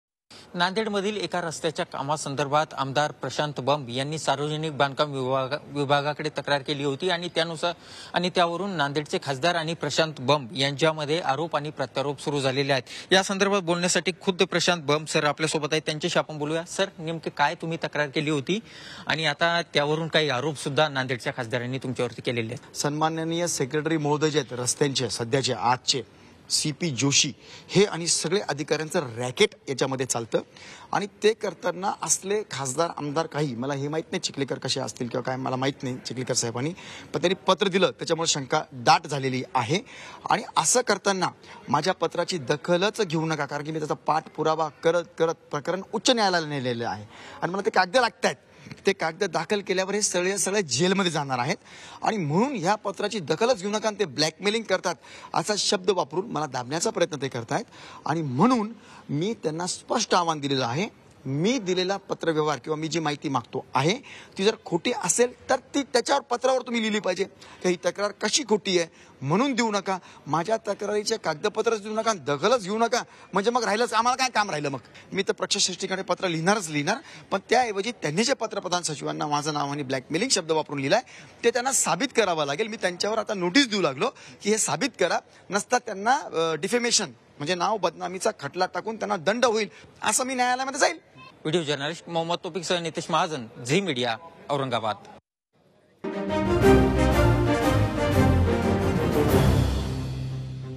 औरंगाबाद | भाजप खासदार - आमदारांतच जुंपली
Aurangabad MLA Prashat Bamb On Allegation By Nanded BJP MP Pratap Patil Chikhlikar